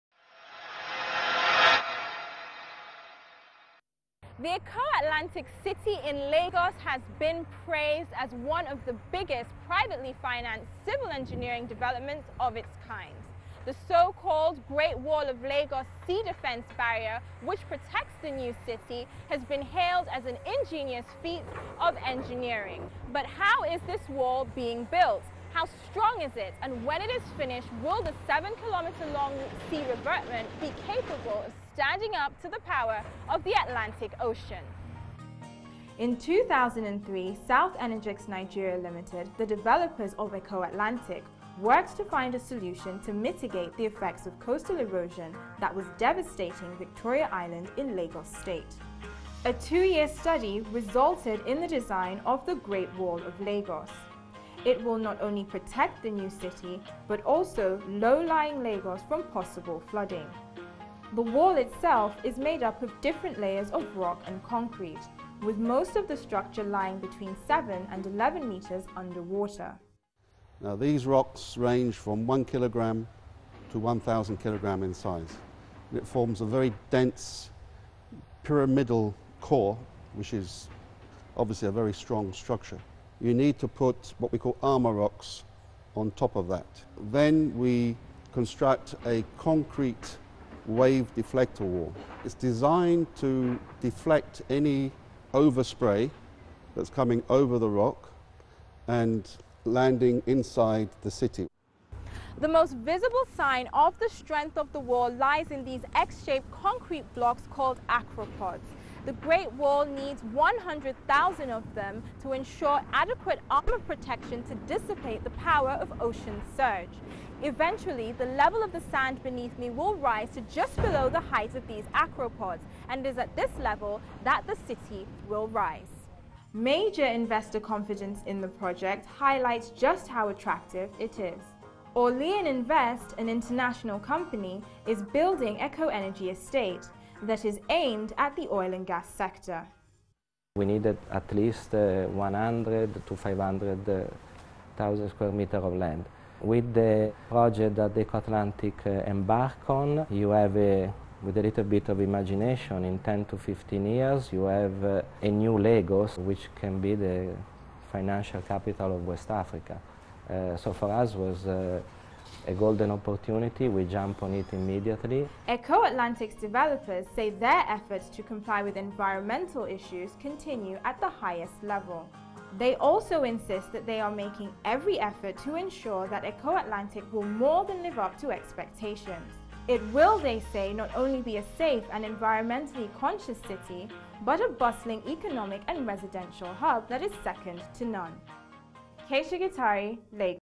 Building the Great Wall of Lagos
Atlantic city in Lagos is being hailed as one of the worlds's biggest, privately financed civil engineering developments of its kind.
The so called Great Wall of Lagos sea defence barrier that protects it has been praised as an ingenious feat of engineering.
But just how is the wall being built? How strong is it? And, when it's finished will the seven kilometer long sea revetment be capable of standing up to the power of the Atlantic ocean?